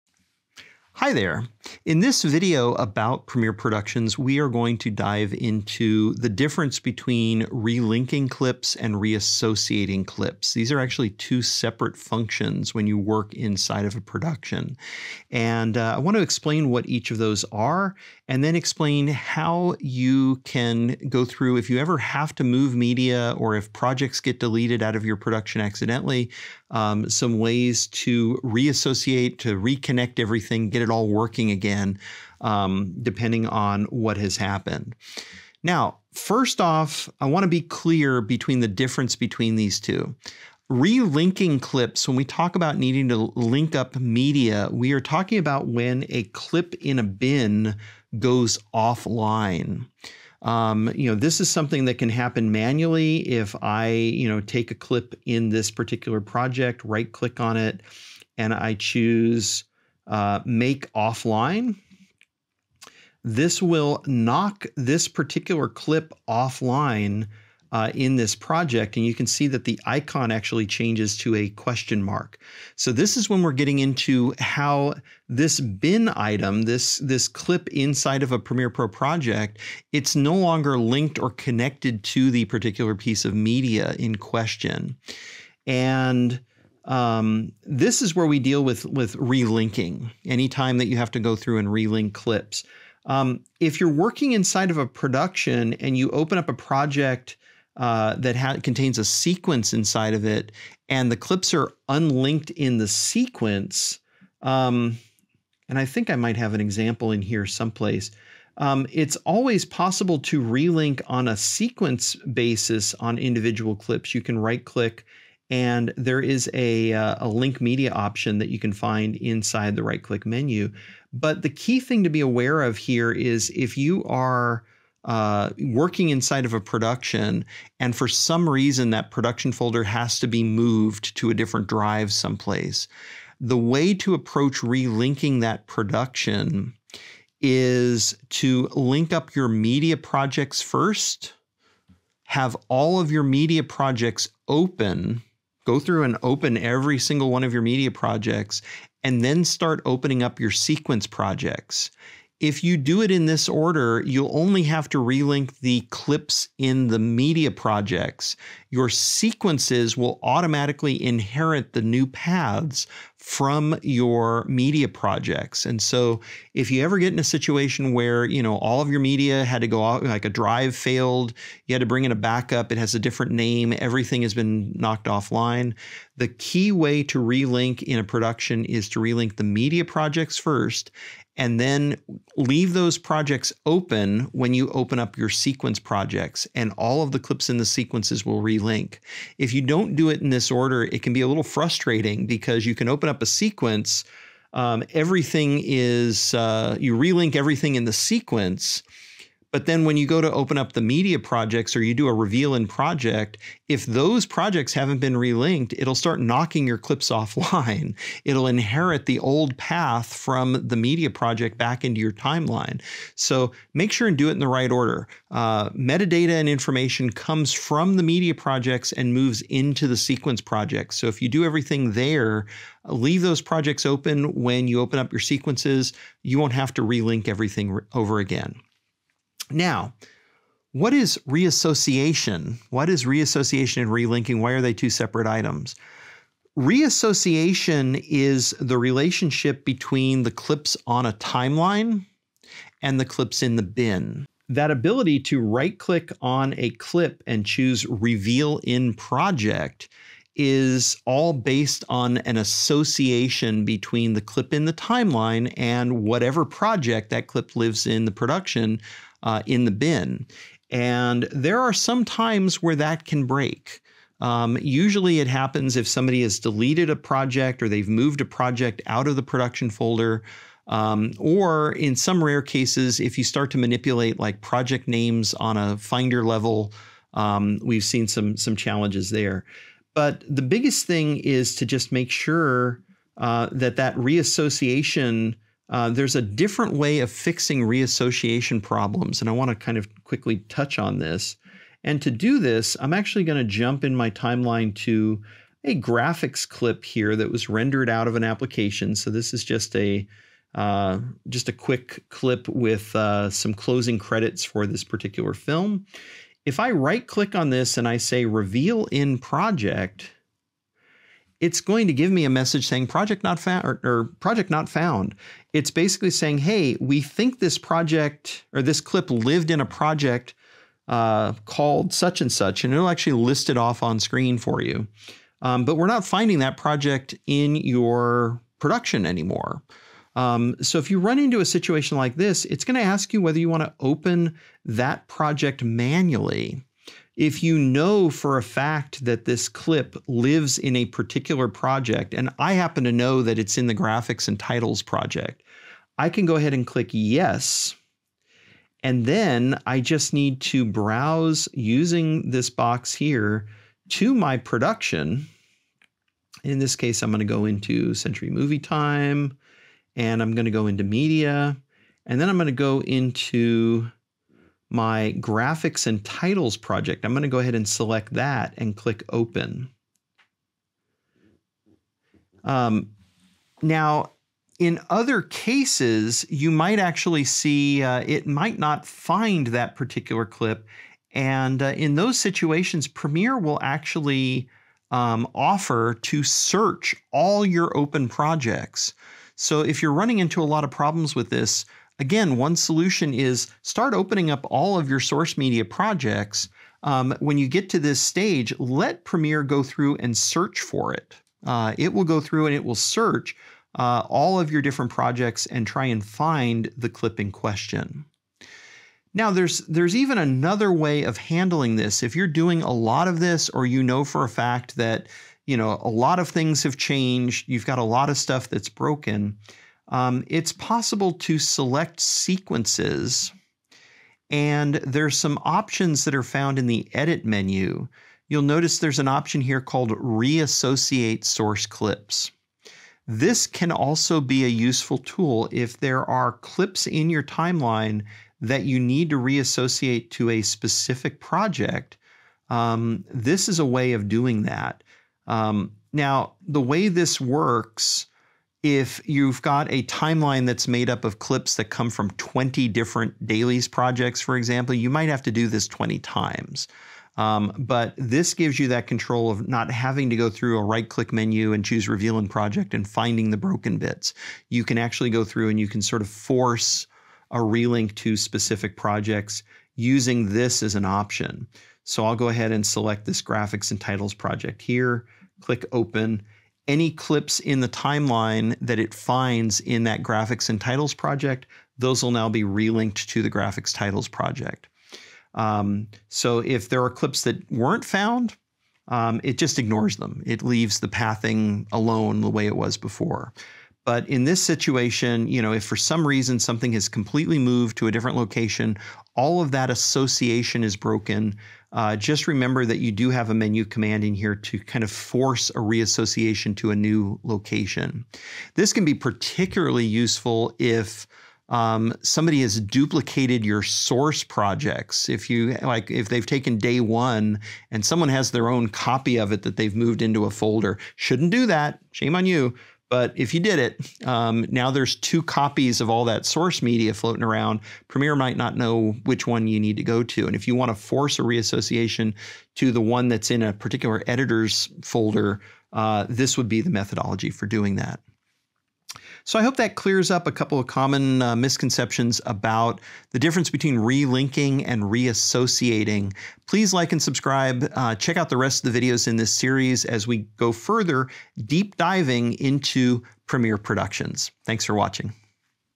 009 Troubleshooting: Reassociating vs Relinking clips in a Premiere Production
Premiere Pro Productions introduce the concept of storing clips and sequences in different projects. What happens when the media becomes unlinked? What happens if the "Reveal clip in Project" command stops working? This video dives into the idea of reassociating clips on a timeline to clips in a bin, and also showcases best practices for relinking media to your clips.